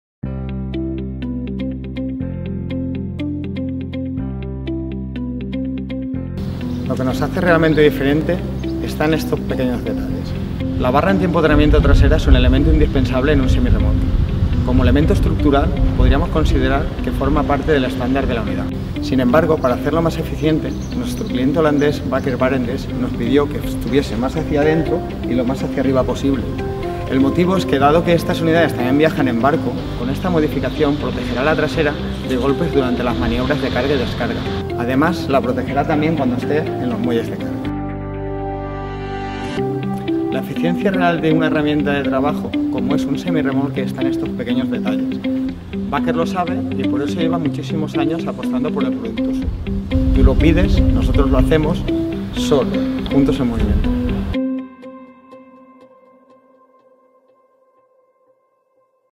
✔ Sor INNOVA. Barra antiempotramiento.
En este video os presentamos la nueva barra antiempotramiento realizada para transporte en barco. La eficiencia real de una herramienta de trabajo está en estos detalles.